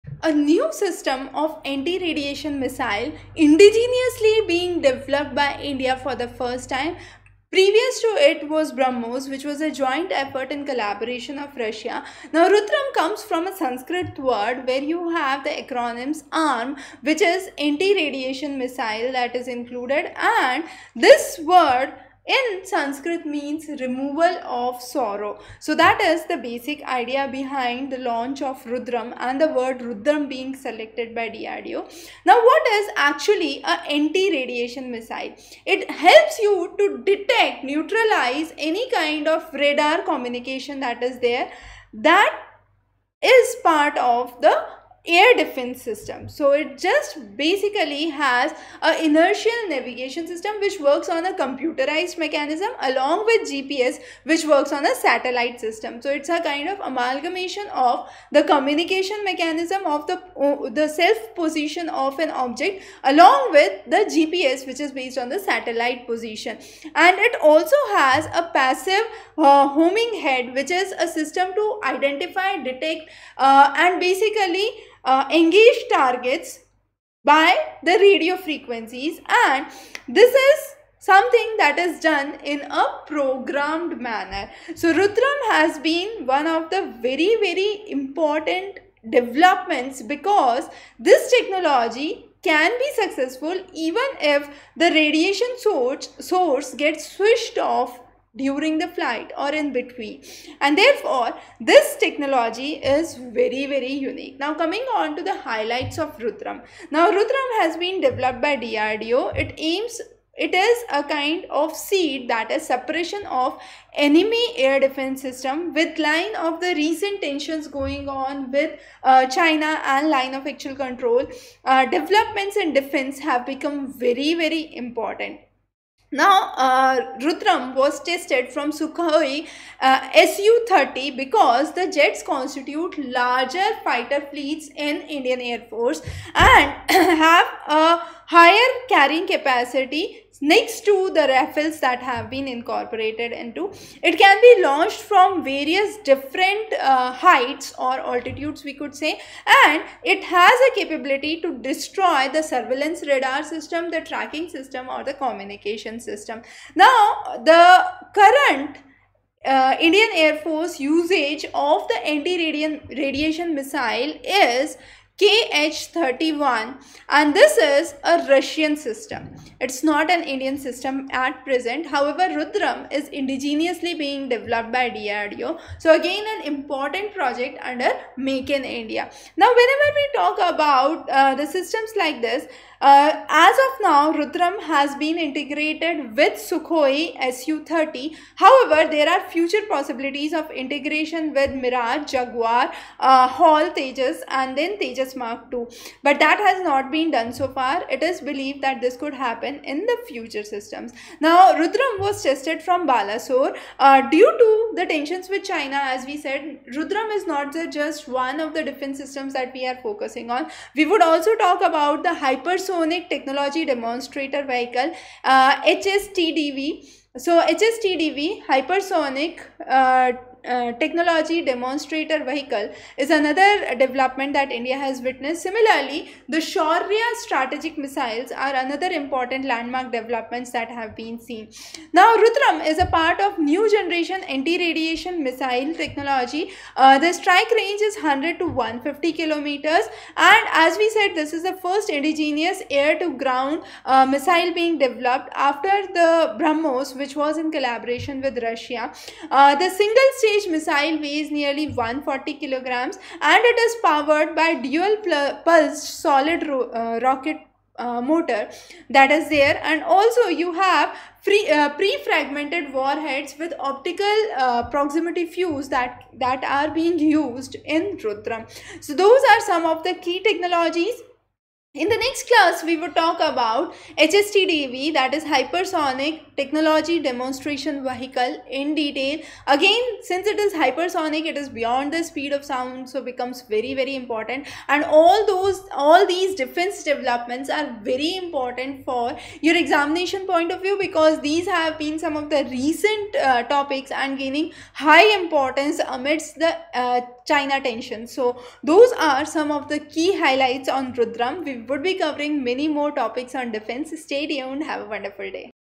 RUDRAM: Anti-Radiation Missile - India's Indigenous Defense Systems | Latest | GS
To detect, track and neutralize the adversary’s radar, communication assets and other radio frequency sources that are part of air defence system
Has inertial navigation system (computerized mechanism that uses changes in the object’s own position) + GPS (satellite based)
Has a “passive homing head”- system that can detect, classify and engage targets (radio frequency sources in this case) over a wide band of frequencies as programmed
RUDRAM can strike accurately even if radiation  sources switch off in between.
Developed by DRDO
Aim for Suppression of Enemy Air Defense (SEAD) system
Can be launched from varying altitude
Can destroy enemy surveillance radar, tracking & communication systems
Can be integrated with Mirage 2000, Jaguar, HAL Tejas and HAL Tejas Mark 2 in future. Currently, with Sukhoi Su-30MKI.
Chapters:
0:00 RUDRAM-Anti Radiation Missile
0:11 Indigenously Developed by India
0:22 BRAHMOS Missile
0:35 Removal of Sorrow is the basic idea
0:46 Air Defence System
1:04 Inertial Navigation System and GPS
1:34: Passive Homing Head
1:55 Developed by DRDO
2:09 Tested from Sukhoi Su-30
2:31 Destruction Power
2:48 Anti-radiation Missile kh-31 Russian System
3:14 Future Integrations
3:29 Tested from Balasore
4:02 Hypersonic Technology Demonstrator
4:30 Sharia Strategic Missiles
4:56 Dual Pulse Solid Rocket Motor